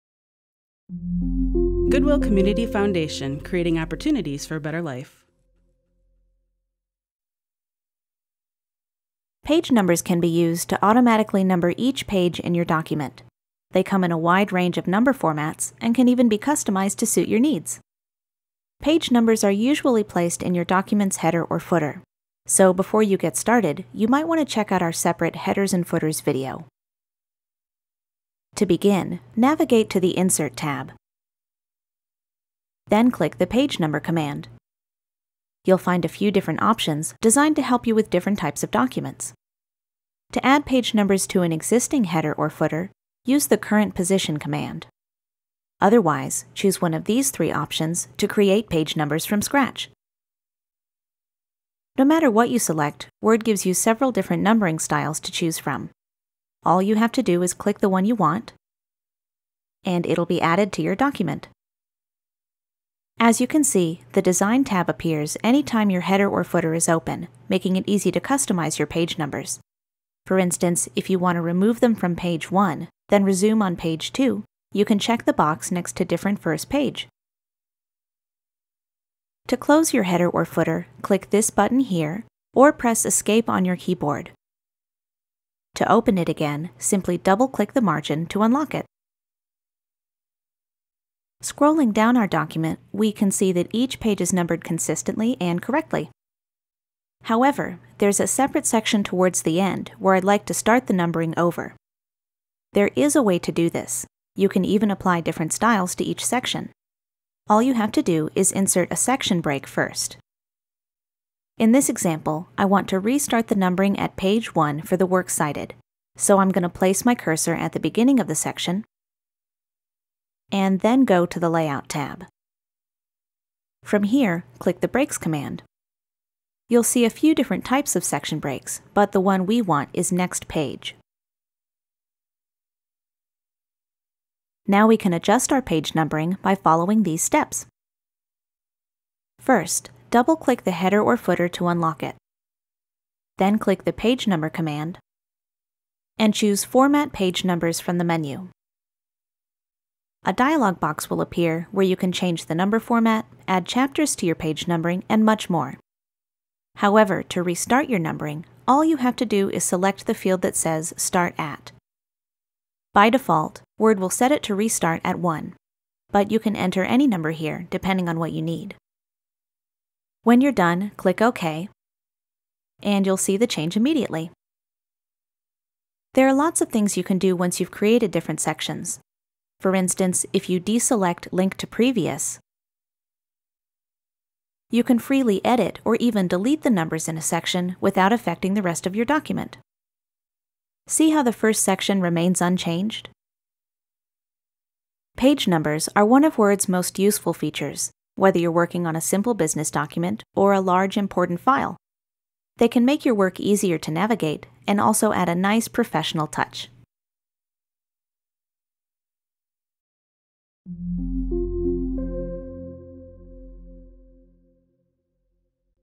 Word: Page Numbers
In this video, you’ll learn the basics of working with page numbers in Word 2019, Word 2016, and Office 365. for our text-based lesson.

This video includes information on:
• Adding page numbers
• Adding page numbers to an existing header or footer
• Hiding page numbers on the first page
• Restarting page numbering